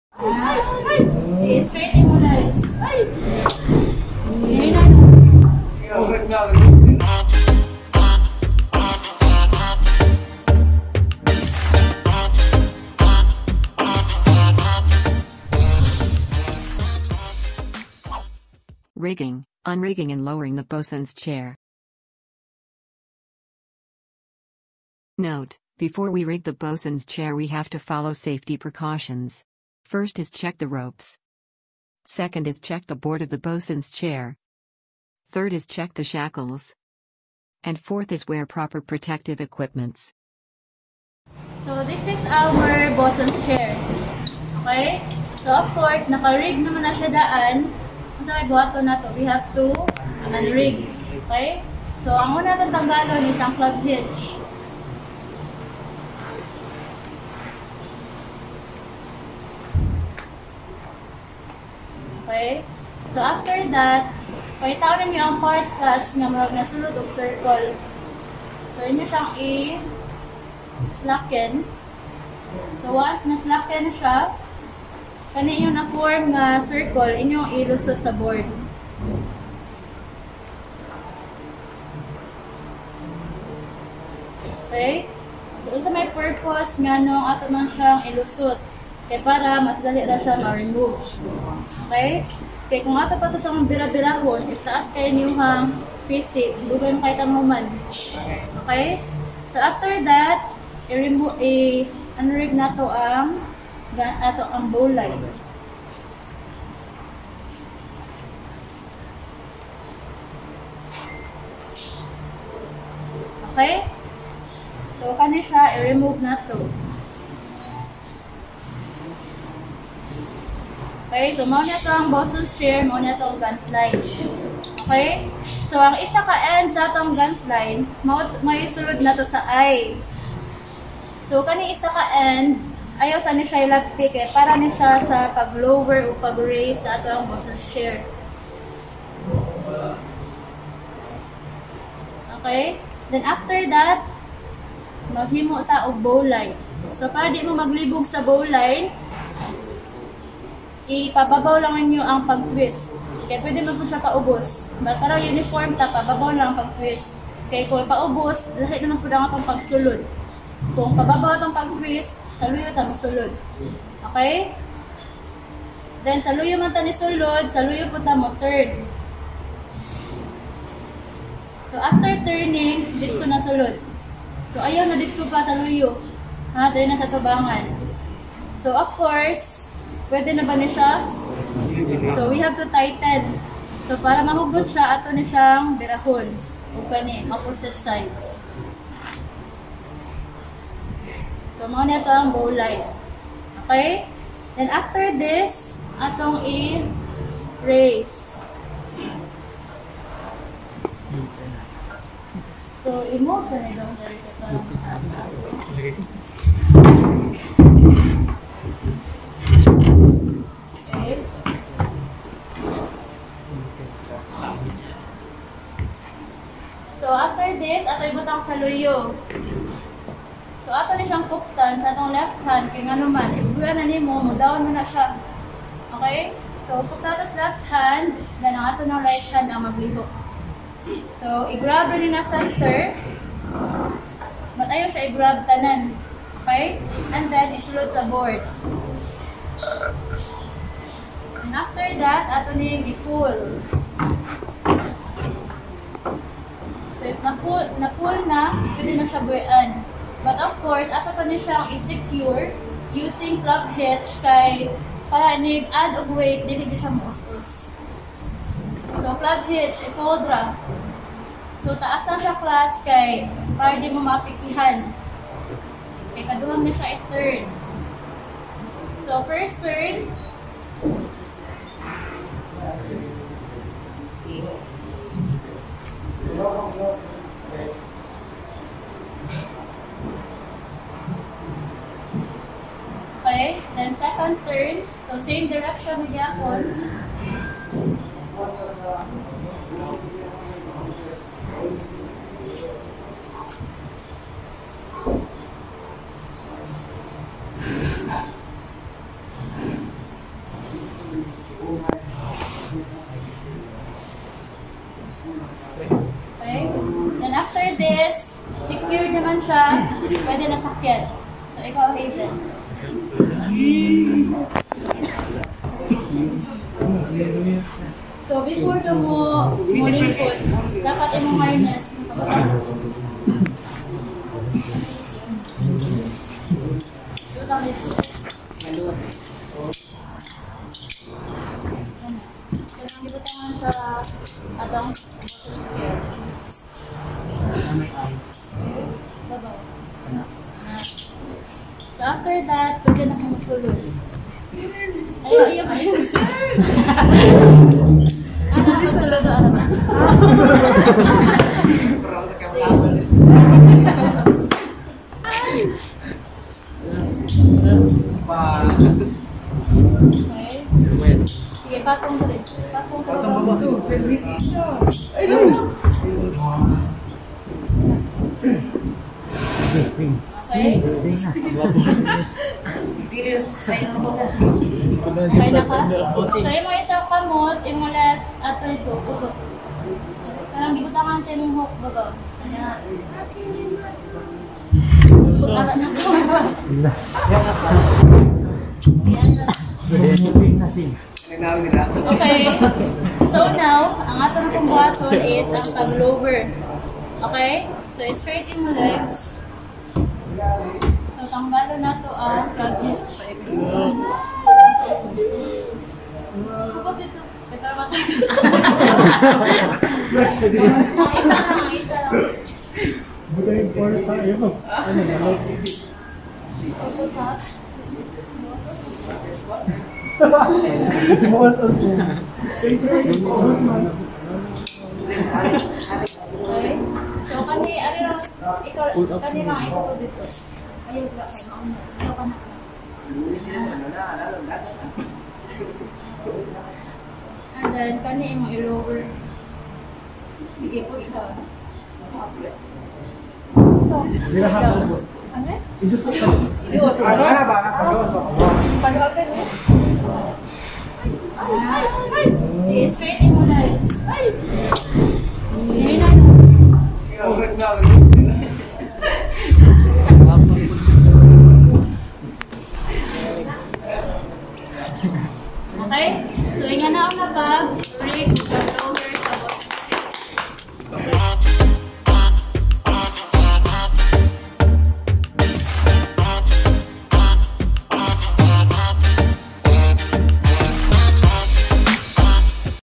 RIGGING, UNRIGGING AND LOWERING THE BOSUN'S CHAIR | @UC-METC LABORATORY
A bosun's chair (or boatswain's chair) is a device used to suspend a person from a rope to perform work aloft. Originally just a short plank or swath of heavy canvas, many modern bosun's chairs incorporate safety devices similar to those found in rock climbing harnesses such as safety clips and additional lines.

In this video, I'll be demonstrating to my maritime students how to rig, unrig and lower the bosun's chair.

This is an important activity to familiarize since not all seafarers get lucky to be an officer on board ship after cadetship. Most of us have to be a rating first to be promoted as Officer.

Working aloft means a work being performed at a height and involving risk of falling, resulting in an injury.

So it's important to all seafarers to be familiar with this one and ofcourse the precautionary measures before rigging the bosun's chair.

I hope you'll learn something in this video.